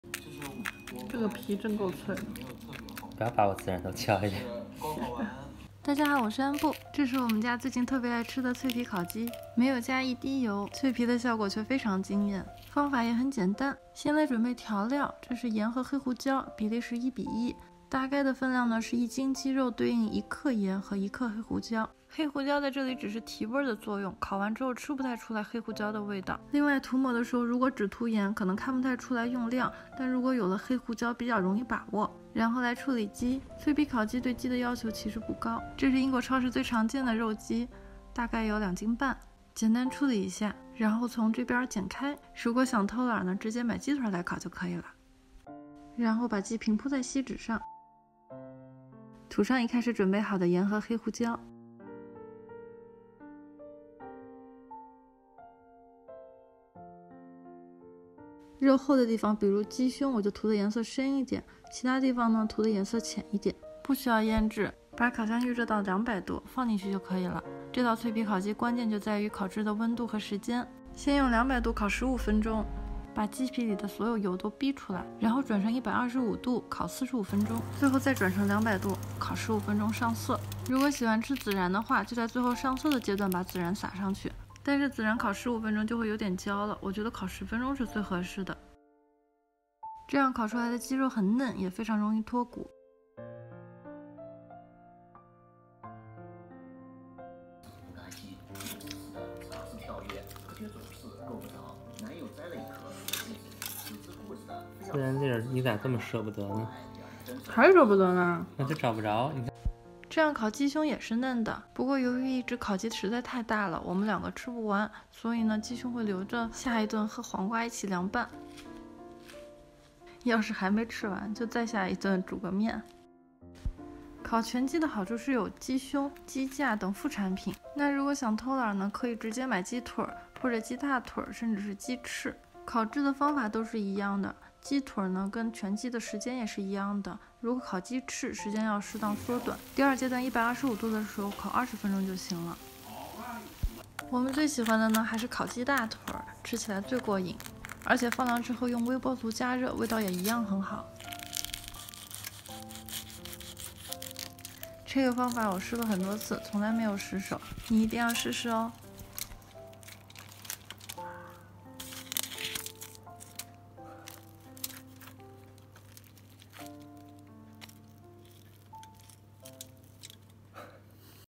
皮！超！脆！烤箱烤全鸡/鸡腿，皮脆肉嫩轻松脱骨，不加一滴油零失手，减肥也能吃
调味料只需盐、黑胡椒、孜然粒（选），掌握好烤箱的时间和温度，在家也能轻松做出脆皮烤鸡，比肯德基的还好吃。吃不完微波炉热一下，跟吮指原味鸡一个味道！（豆爸的评价）